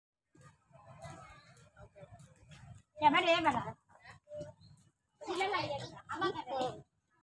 Sculpture wild animals
Mid autumn festival at takashimsya every year they celebrate the lunartic you can see and try to taste the different flavors of moon cake . the biggest Fair at takashimaya enjoy watching..